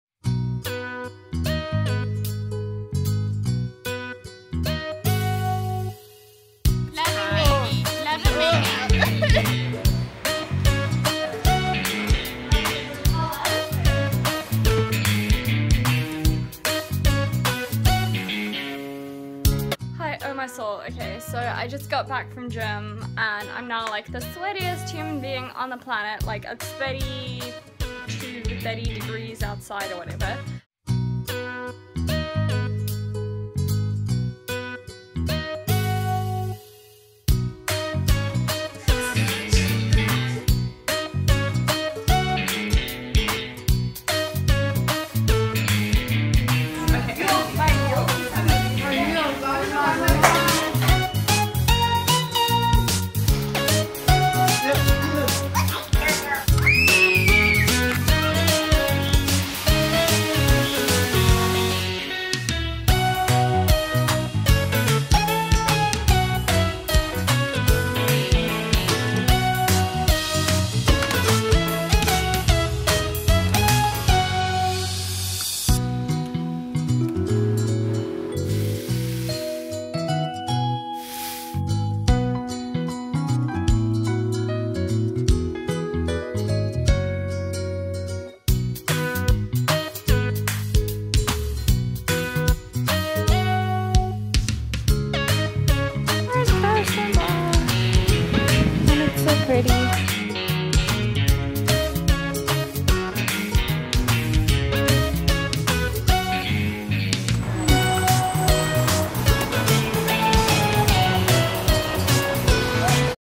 Family Vibes & Kirstenbosch
Fun day with the family, spending time with my friends at Kirstenbosch Botanical Gardens and eating lots of random yummy things during the week. :)